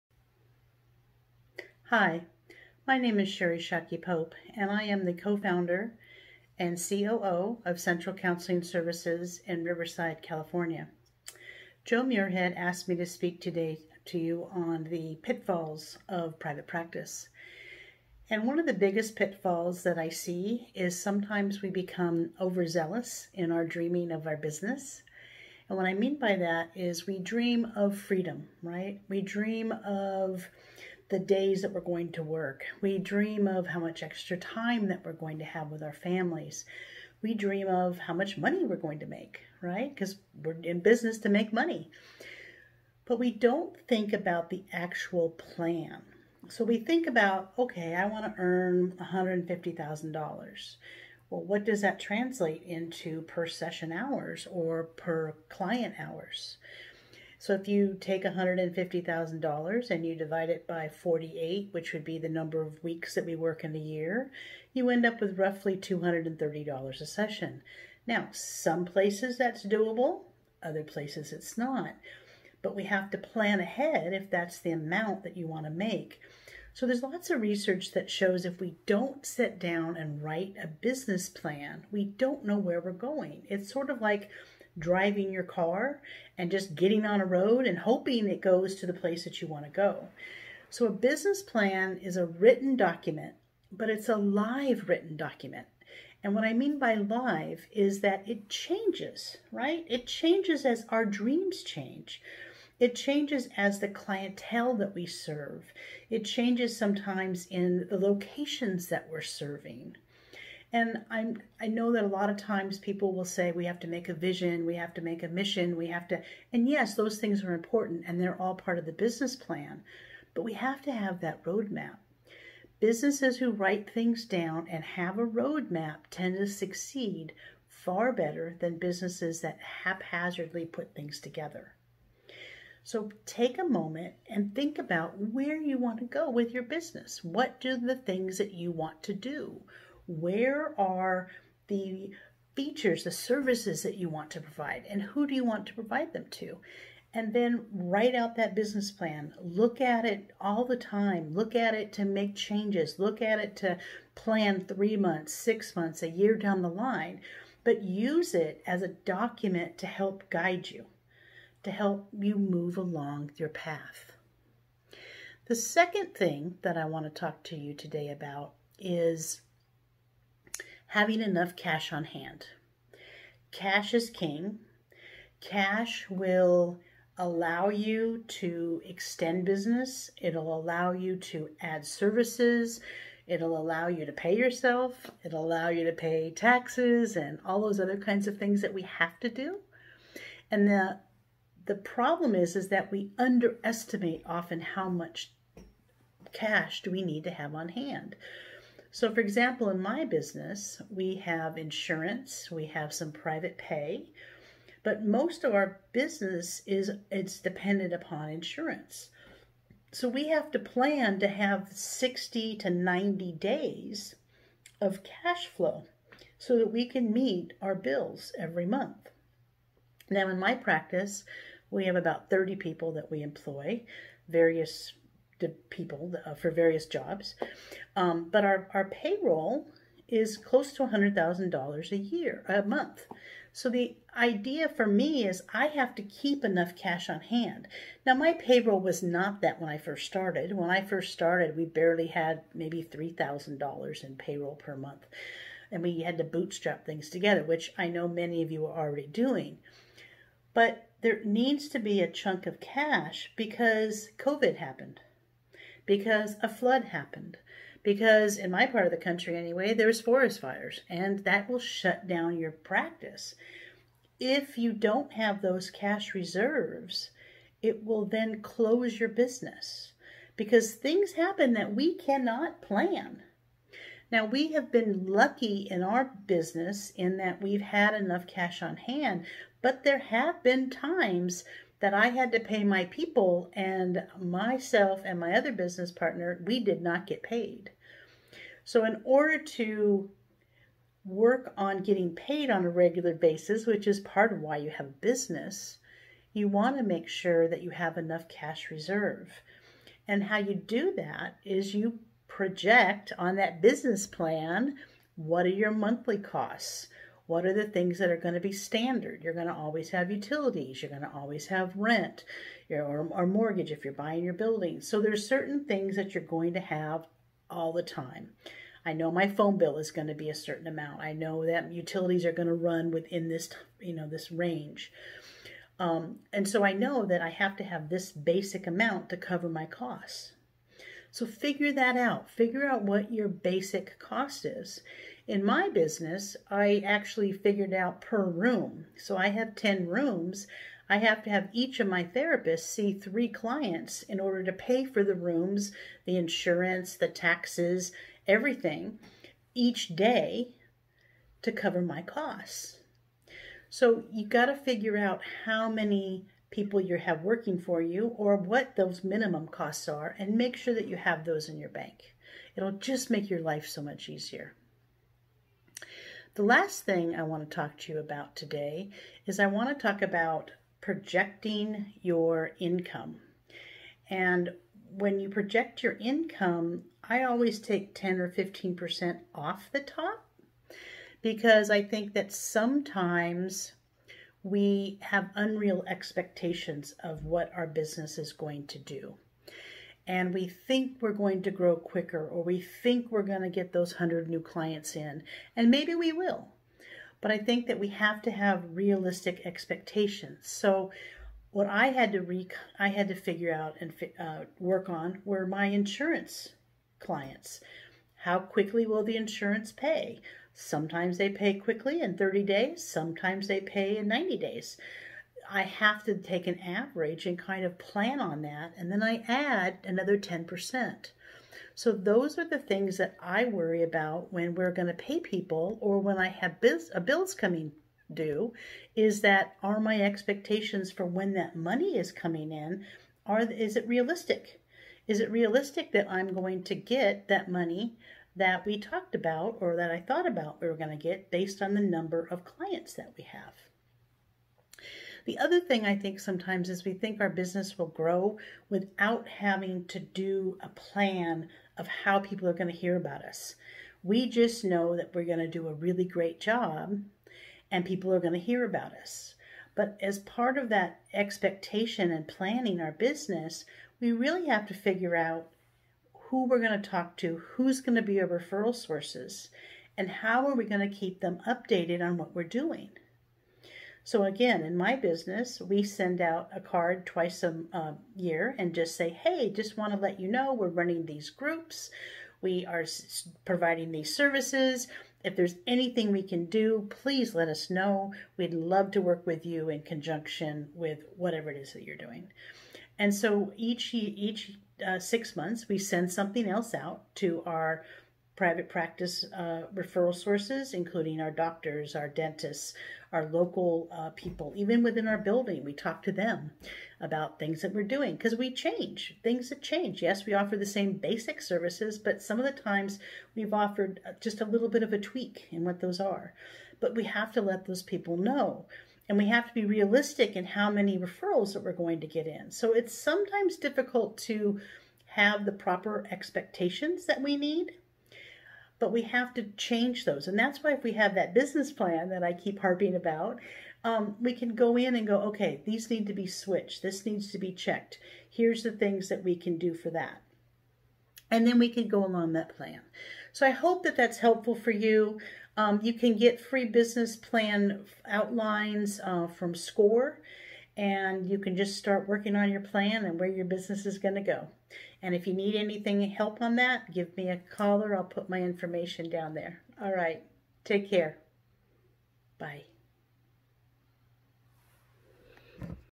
Episode 402_This Is Private Practice with Sherry Shockey Pope
I have another fantastic guest for you today. Sherry is a wise woman. She has the smarts in running a business that so many of us wish we had

In this episode, Sherry shares with us

common pitfalls when running a Private Practice
How Math makes us $$$
Why a business plan is more than a piece of paper
How CASH is king
and how we like to overemphasize what we will achieve

THANK YOU SHERRY this was excellent

PS Happy weekend everyone